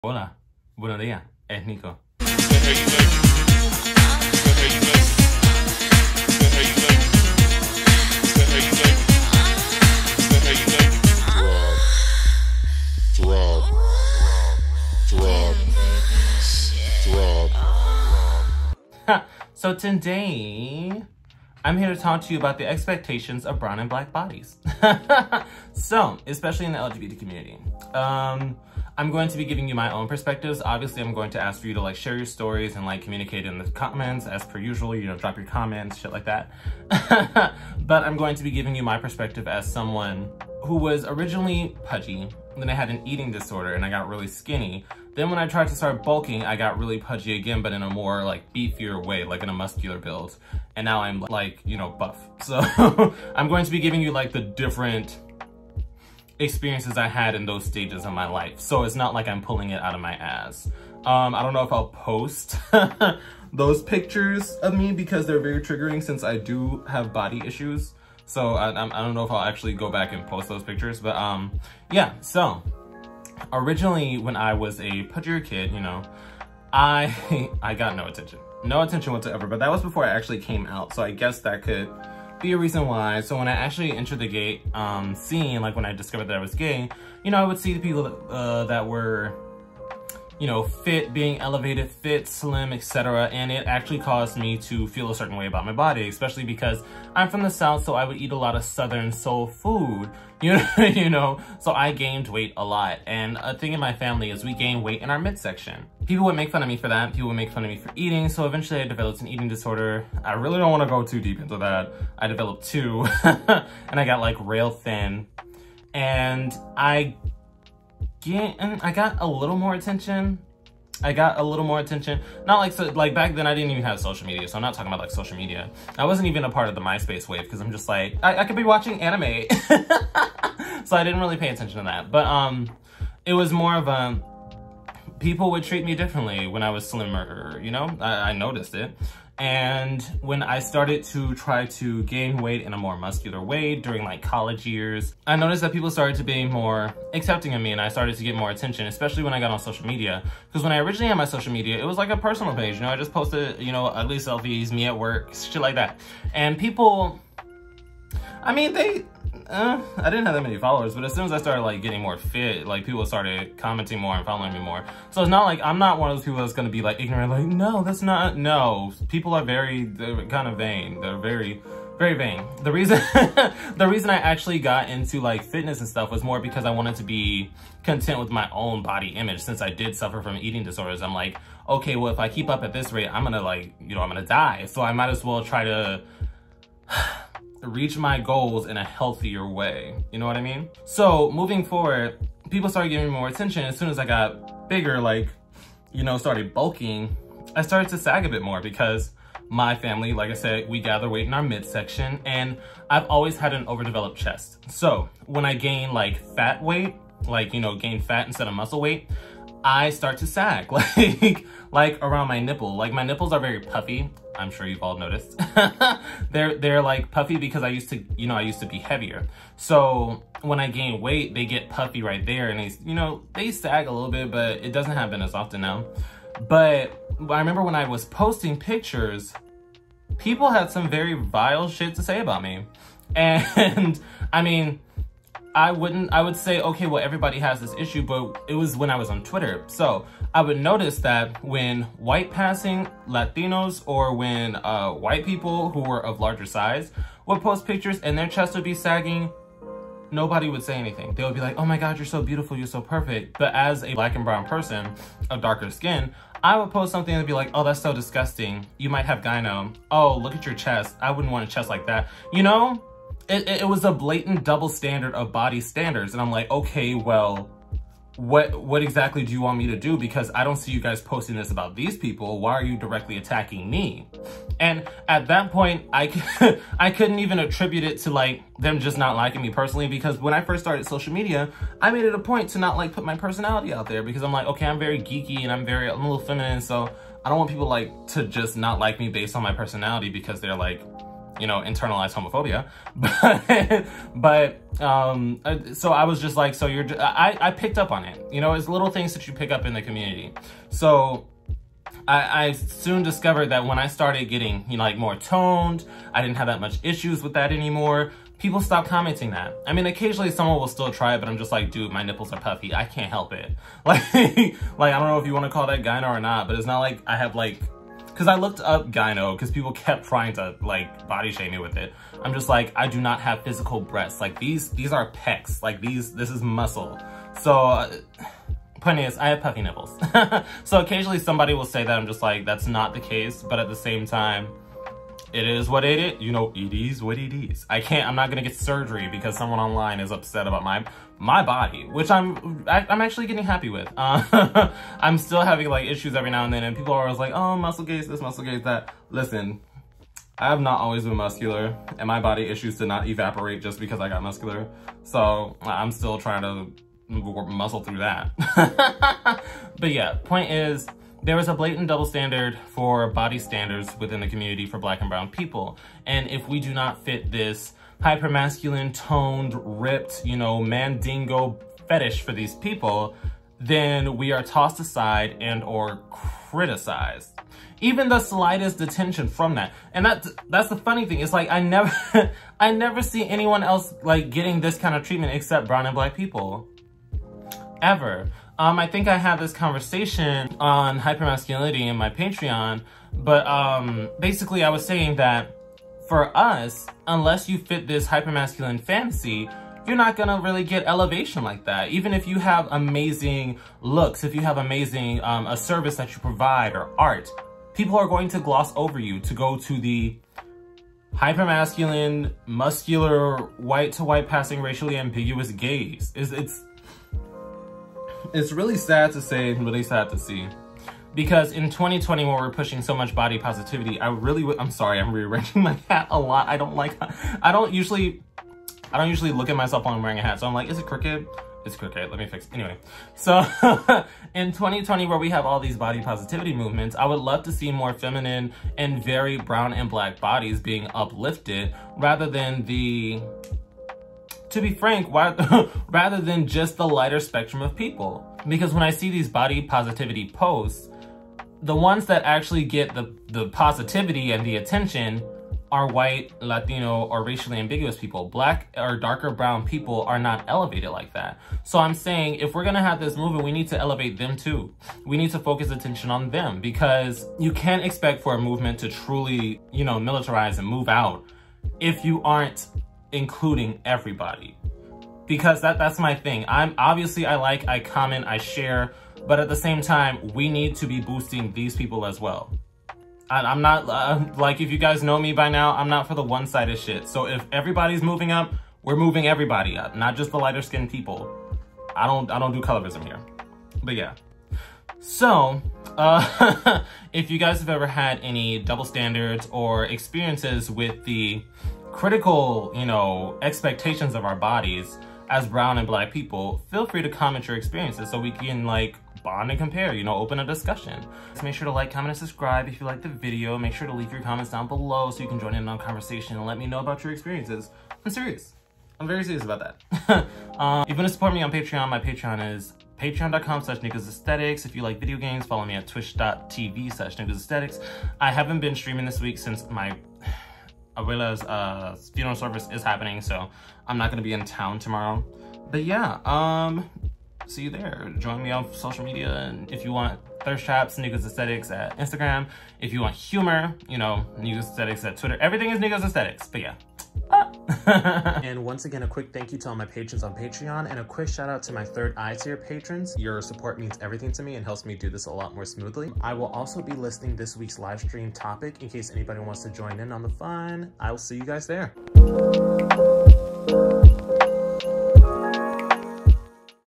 The Expectations of Brown & Black Bodies In The LGBT Community
A brief discussion on the exclusion of brown & black bodies in the LGBT Body Positive Scene.